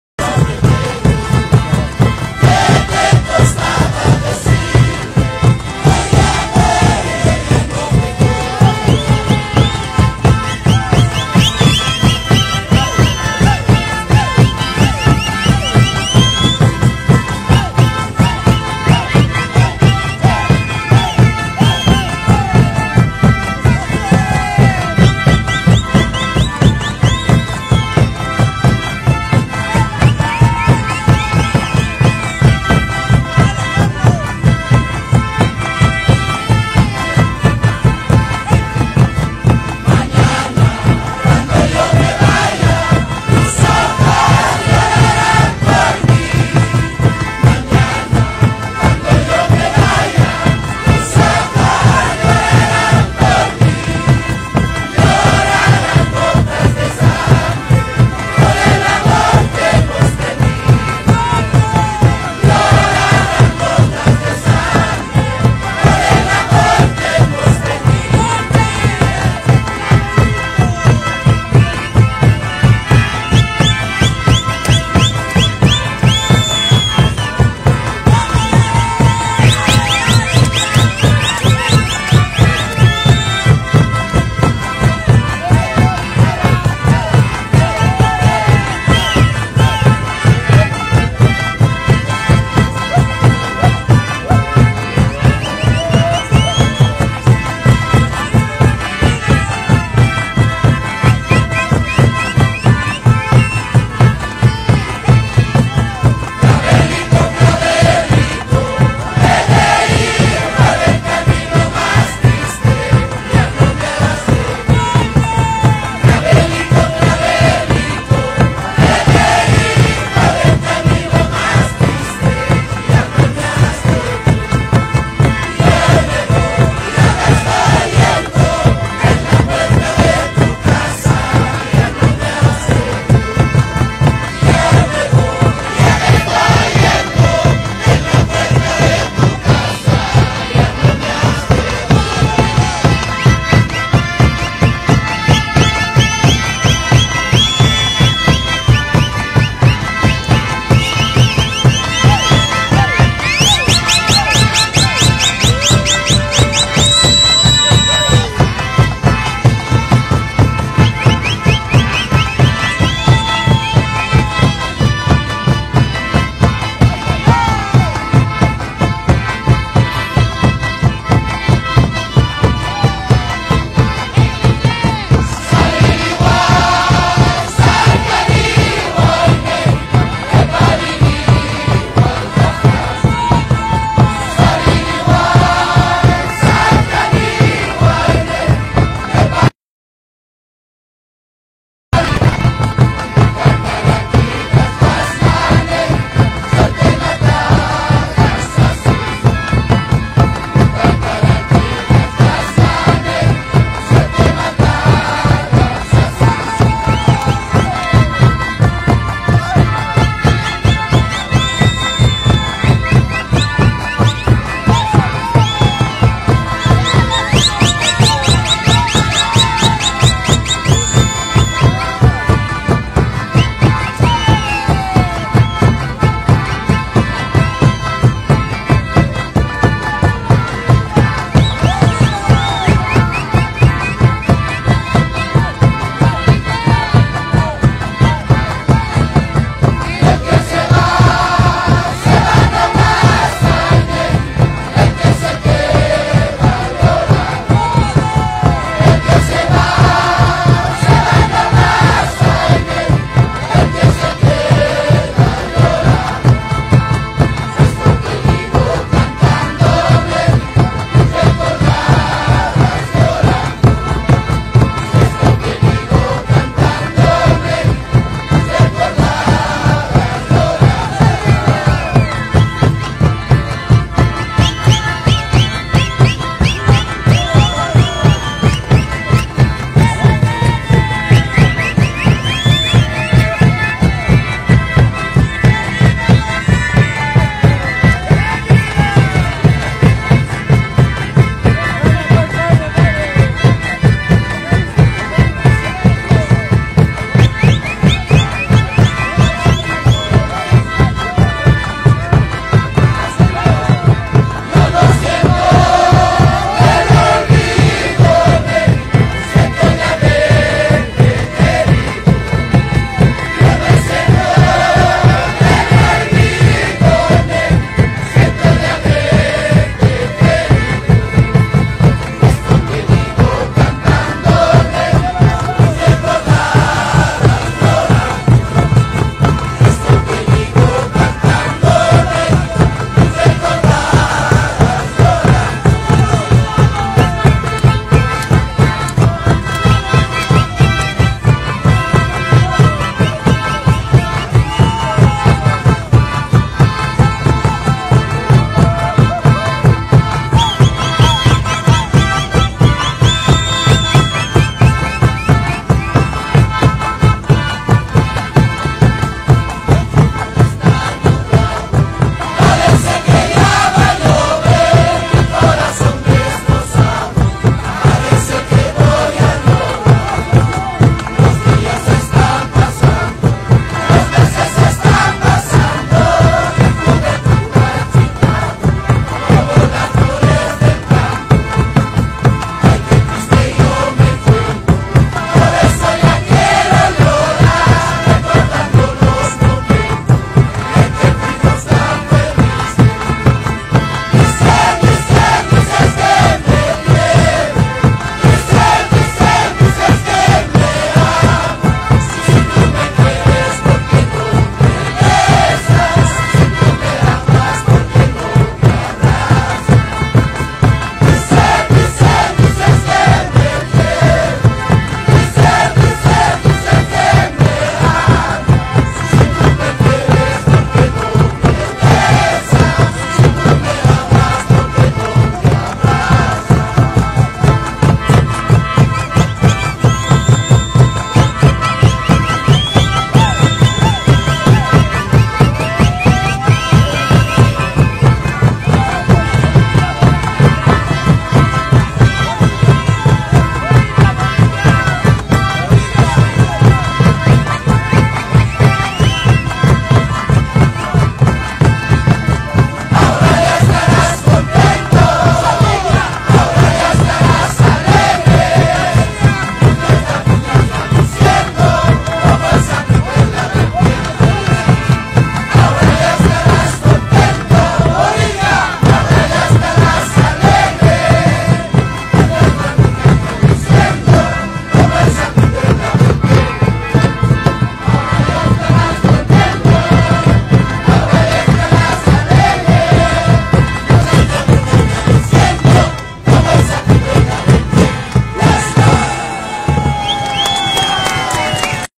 LOS PUKARAS 2020 PLAZA MURRILLO LA PAZ BOLIVIA CON SU MEJOR MIX DE MUSICA FOLKLORICA (en VIVO)
LOS PUKARAS 2019 PLAZA MURRILLO LA PAZ BOLIVIA CON SUs MEJORES MIX DE MUSICAS FOLKLORICA
Muchas Felicidades en los 45 años de existencia, del grupo Folklorico, el género Pukara aportando significativamente en la cultura boliviana con las composiciones y grabaciones musicales.
HOY EN DIA LA ASOCIACIÓN PUKARA PREVÉ FORTALECER EL GÉNERO MUSICAL COMO UNA EXPRESIÓN PATRIMONIAL DE BOLIVIA